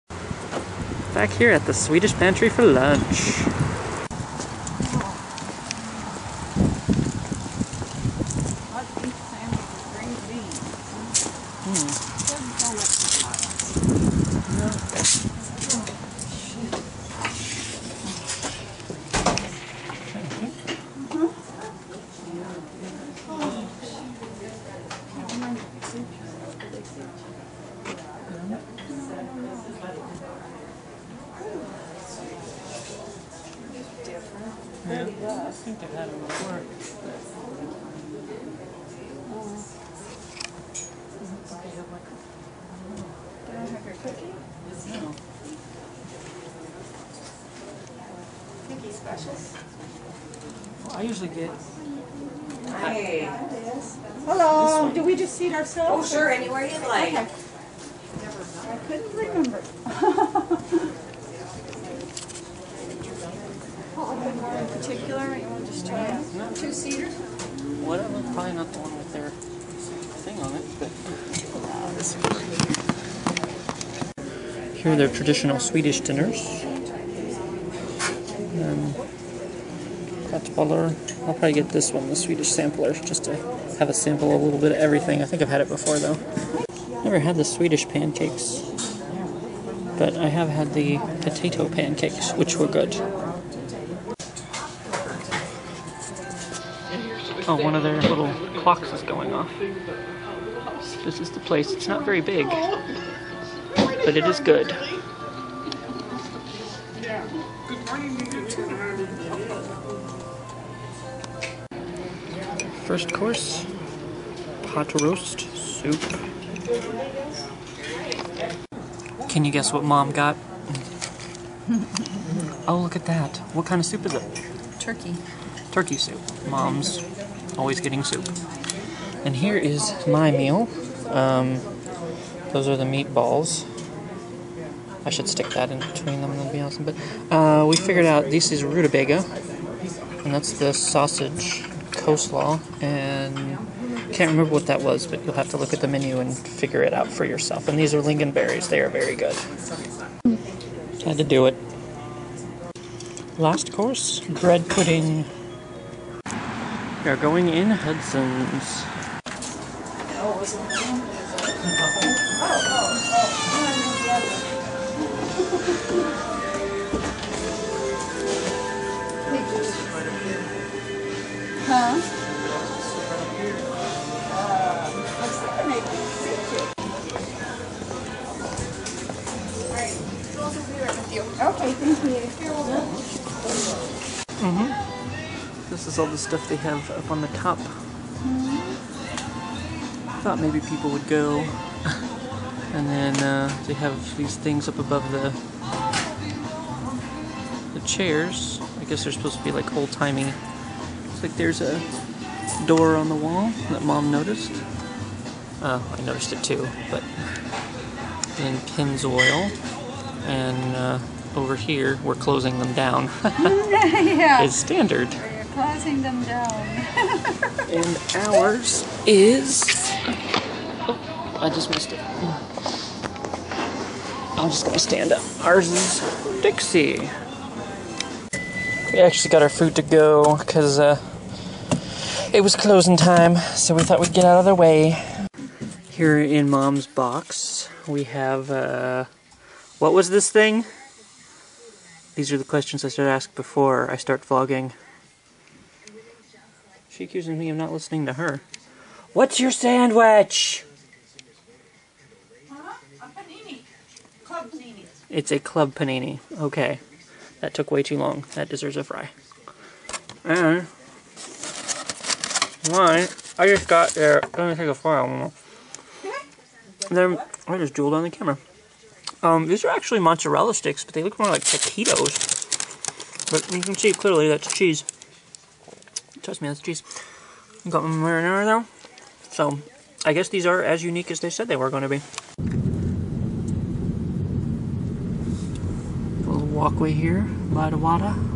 Vacaday Got A Bonus! (Day 6: Food)
Martheev and Mom have some good munchin's in Escanaba Michigan.